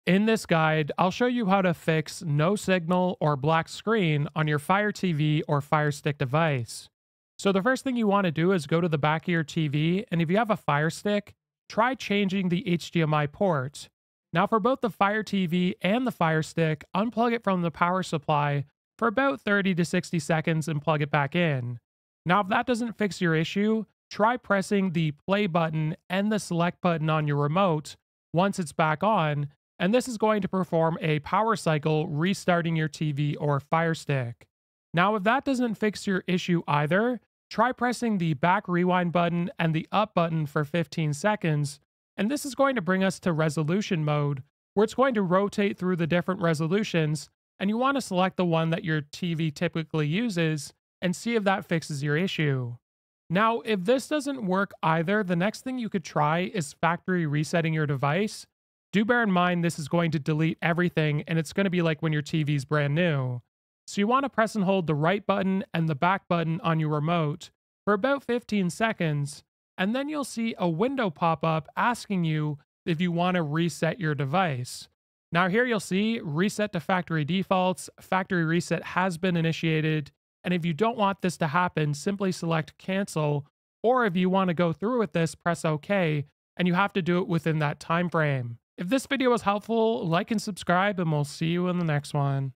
How to Fix No Signal or Black Screen on Fire TV or Firestick!
How to fix no signal or black screen on Fire TV or Firestick? In this guide, I show you how to fix no signal or black tv screen on your Fire TV or Firestick.